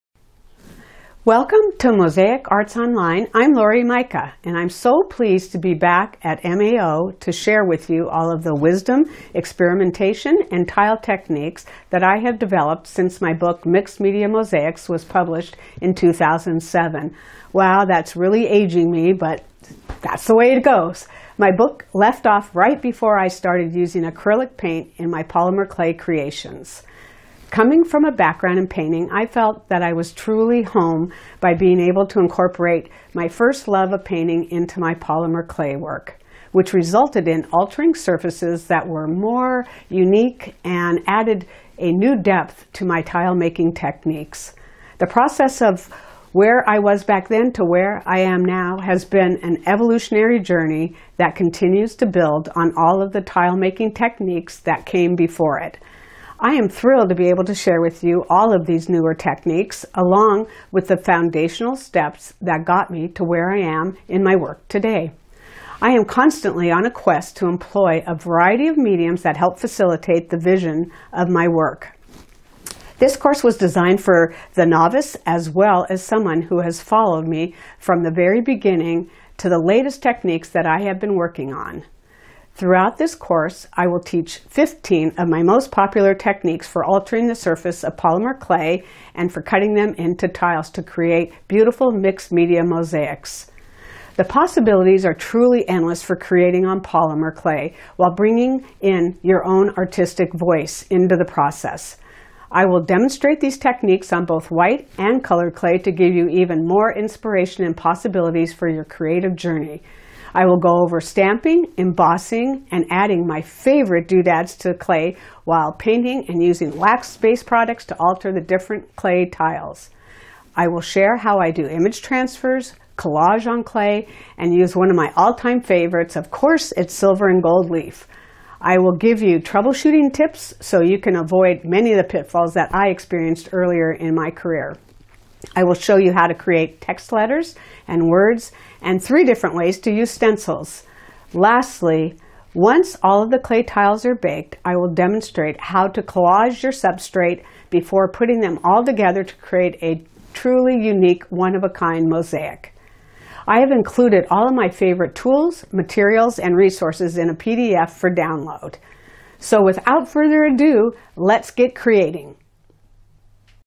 Handmade Polymer Clay Tiles for Mixed Media and Mosaic Art
Take your polymer clay making to a whole new level with multiple mixed-media and mosaic techniques

Have you ever looked at intricate polymer clay pieces and wondered how to incorporate them into your mosaic artwork? Are you curious how to infuse your mosaic projects with a unique touch that reflects your personal style?

Look no further – Laurie Mika, our renowned Mosaic Arts Online artist, is thrilled to guide you through this exclusive Mosaic Arts Online course that promises to elevate your polymer clay work to new heights.

Laurie's distinct approach to altering the surface of polymer clay into art has become her signature style, and in this course, she generously shares a wealth of techniques.

Whether you're a seasoned artist or this is your first dive into the world of polymer clay art, Laurie's insights empower you not only to replicate her methods but also to seamlessly integrate them into your own mosaic or mixed-media endeavors.

If you're unfamiliar with Laurie's work, she's the creative mind behind the book "Mixed Media Mosaics." Although she hoped to write a follow-up book instead, Laurie has taken that energy and poured it into the content of this course, providing you with instant access to a multitude of content and the techniques she has developed since that time.

Here's some of the content you will learn in the fun world of polymer clay tile making for mosaic art:

*Laurie’s favorite tools and materials
*Clay conditioning techniques
*Painting and scoring tiles
*Stamping and collaging on white clay
*Laurie’s signature image transfer technique
*Adding text to white clay
*Incorporating stamps and texture on a variety of colored clays
*Creating a central focal point
*Using gold and silver leaf for an aged effect
*Stenciling and spray paint techniques
*Crafting words with stamped letters
*Bringing it all together to craft a unique mixed-media mosaic project

This comprehensive course gives so much insight into the world of polymer clay mosaics. Laurie shares countless ideas and techniques, ensuring you acquire all the knowledge and confidence needed to create your unique artistic vision.

Included in this video course is an in-depth list of Laurie’s favorite tools, materials, and resources for you to choose from to download.